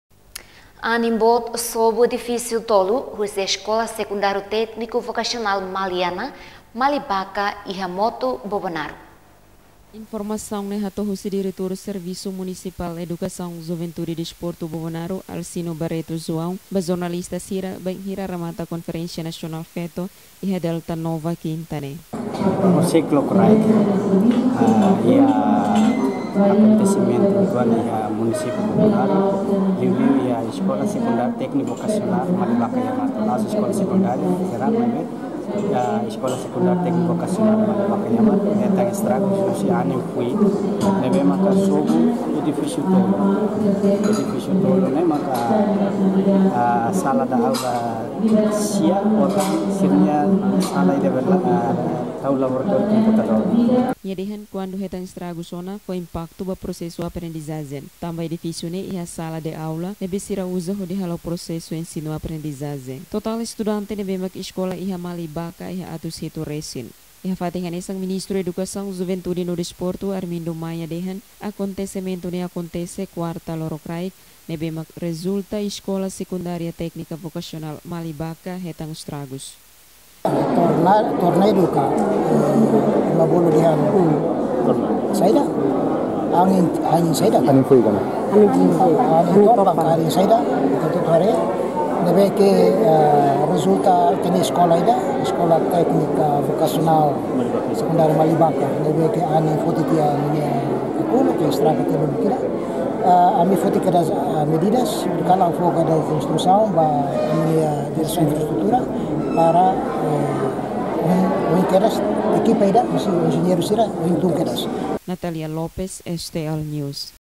Anin Boot Estraga ESTV Bobonaro (News 03-12-2020)
Anin Boot Sobu Edifisu Tolu, Husi Eskola Sekundariu Tekniku Vokasional Maliana, Malibaka, Iha Mato Bobonaro.

STL News Kalan, 03 Dezembru  2020